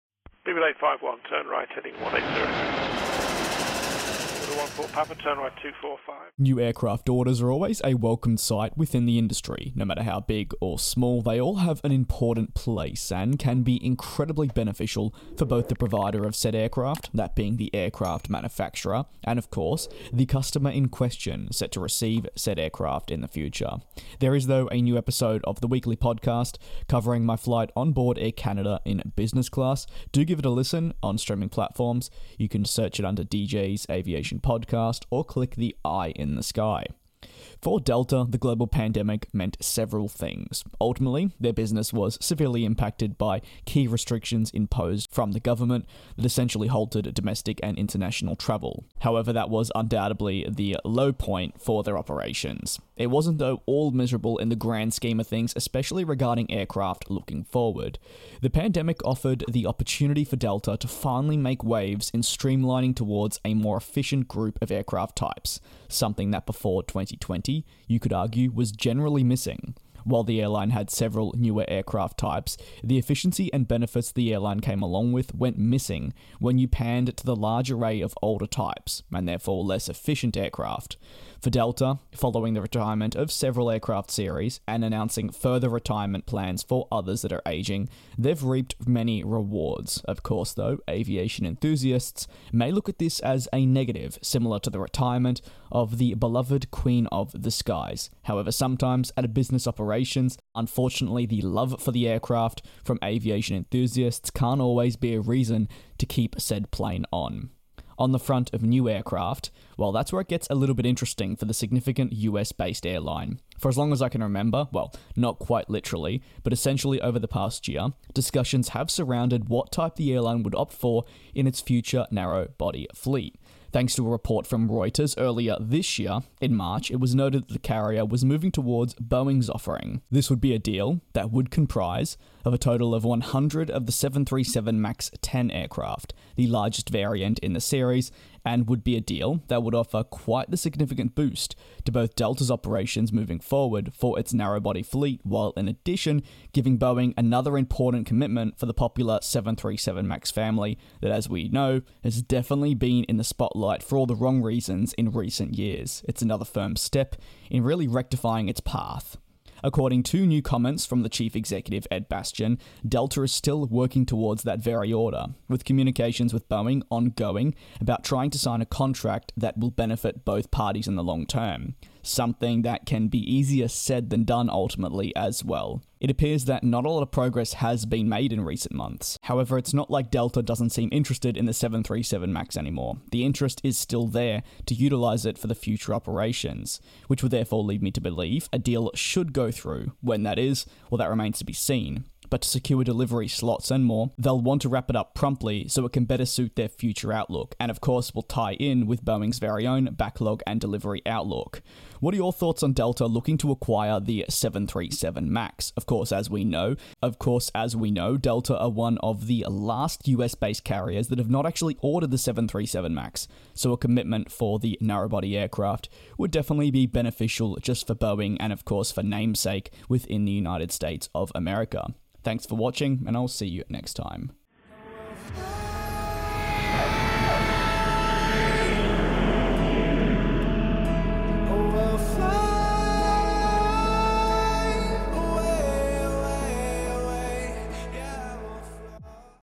Delta To ORDER New AIRCRAFT?
Delta is eyeing the purchase of new aircraft within the aviation industry from American aircraft manufacturer Boeing. Today I explore how the pandemic impacted Delta, from the positives to the negatives, with a critical focus on aircraft types with the retirements of ageing aircraft and the streamlining of operations.